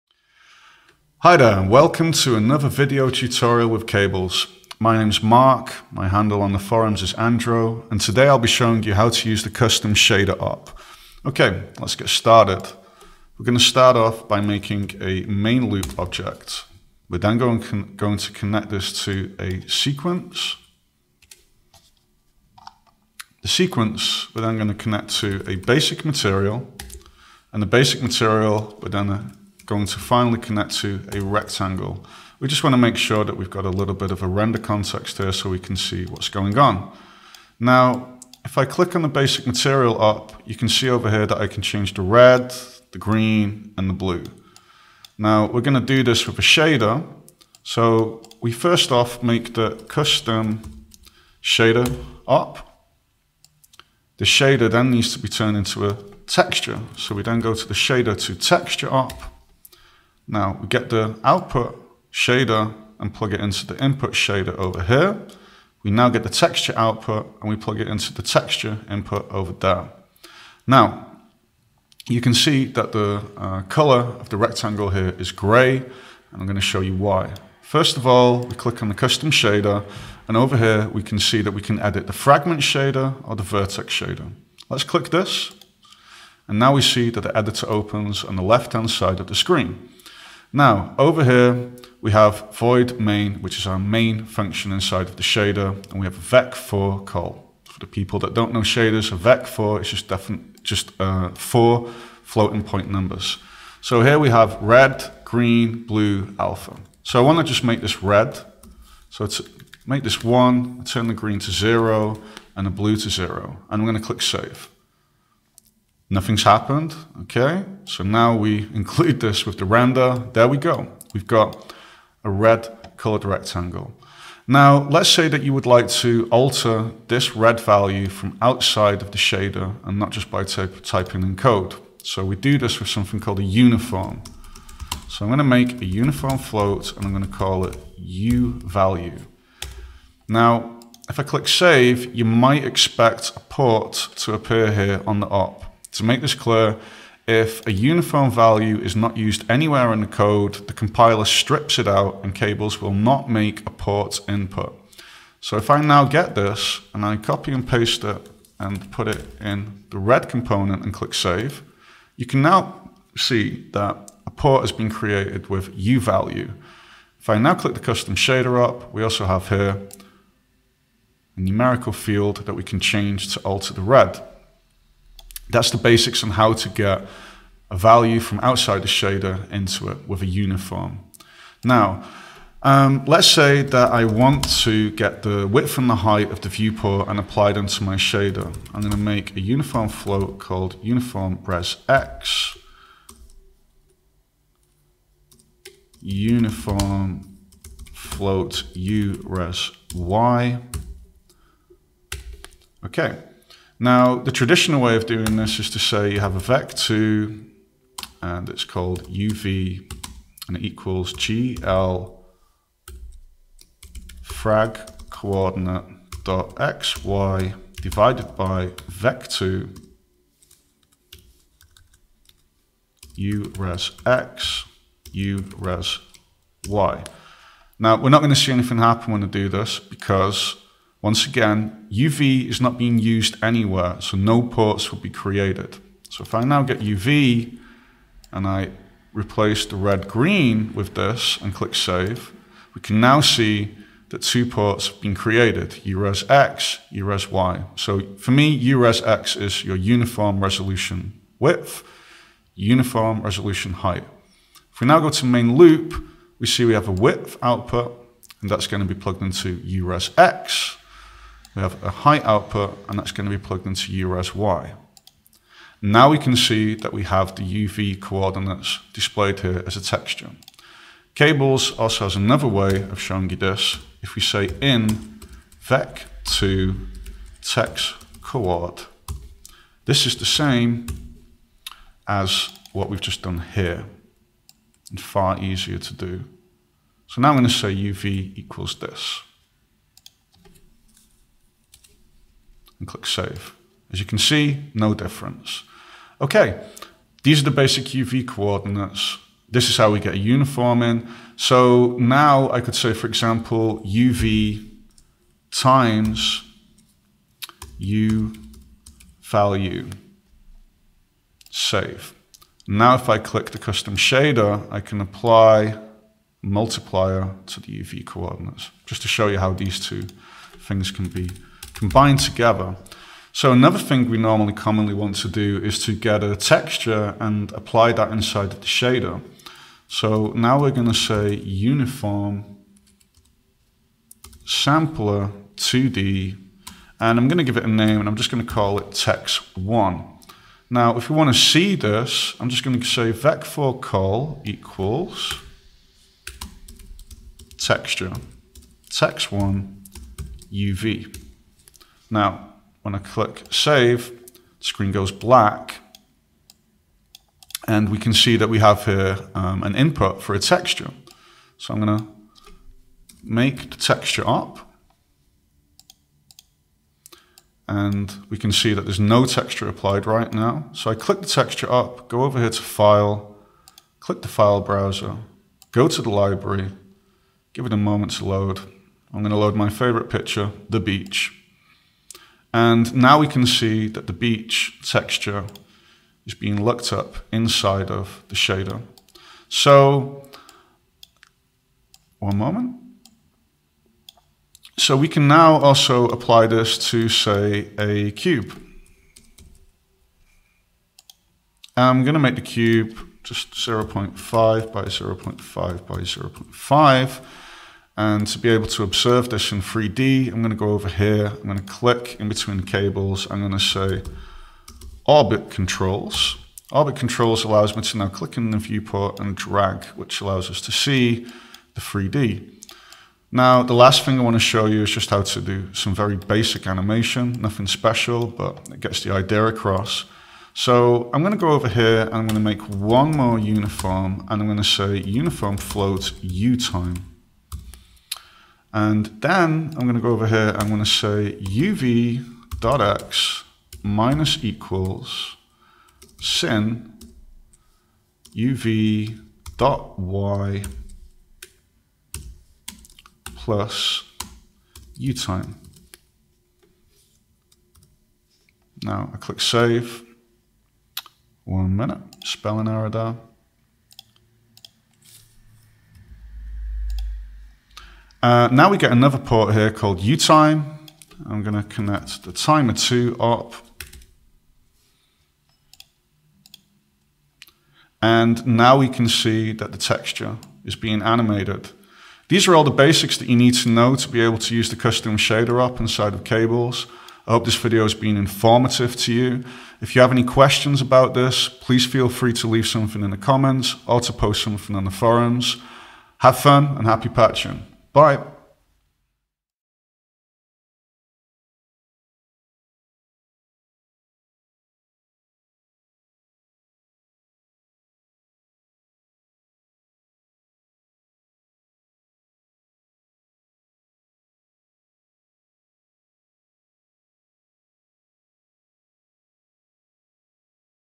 Coding with cables - custom shader op
This video will show you how to get started with the custom shader op in Cables.